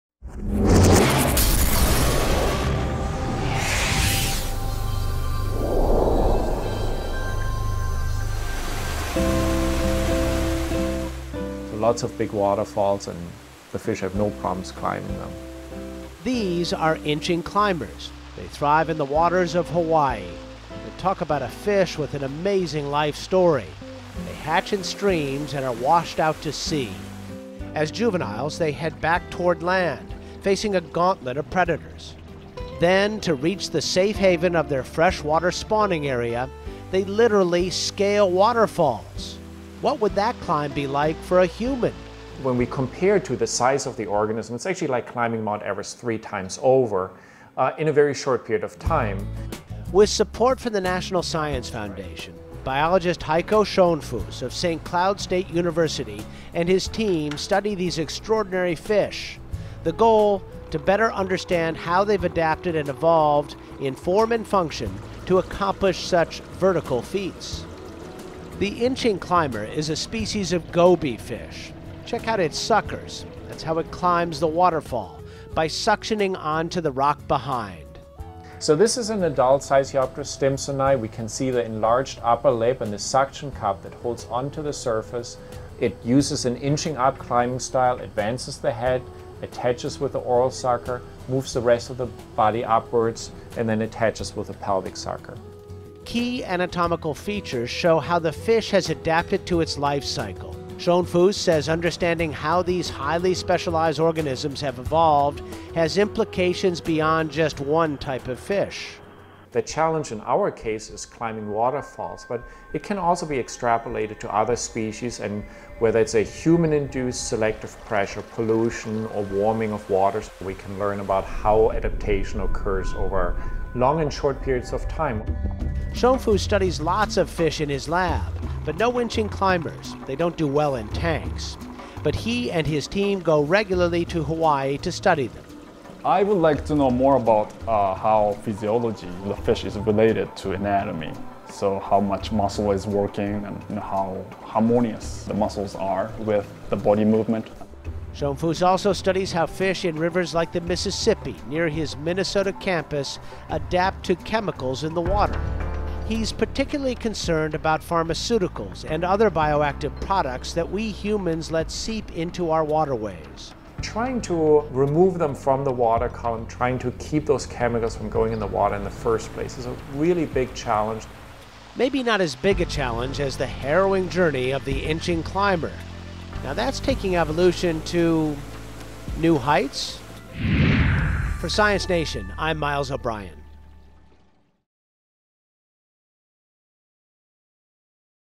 Un pez que escala montañas, como si tu subieras tres veces al Everest
La especie Sicyopterus stimpsoni es endémica de las islas Hawái.
El tamaño de estos animales en su juventud no suele ser mayor de unos pocos centímetros, pero es capaz de escalar cientos de metros de altura. Si lo comparamos con el ser humano es como si éste escalara una montaña que tuviera tres veces la altura del Everest.
Cuando llegan a la juventud se desplazan del mar a los arroyos de agua dulce de las islas. Poco antes de comenzar su camino se producen cambios en su boca que hacen que los animales no puedan alimentarse con el plancton marino, los dientes cambian de posición desplazándose a la parte inferior de su cuerpo. Esto ocurre dos días antes de comenzar la escalada. En ese momento para alimentarse necesitan llegar a las algas que crecen en las partes altas de los arroyos de montaña.
En estos lugares las hembras desovan y los machos fertilizan los huevos que son rápidamente arrastrados por las aguas al mar donde se repite su ciclo natural.
Para llegar a las aguas dulces estos peces deben sortear altas cascadas que escalan subiendo por las piedras que hay detrás o a los lados; para ello se sirven de las ventosas que hay en su cuerpo.
En este vídeo, realizado por la National Science Foundation para estudiar la capacidad adaptativa de esta especie, puedes ver más detalles de la anatomía y forma de escalar de este singular pez.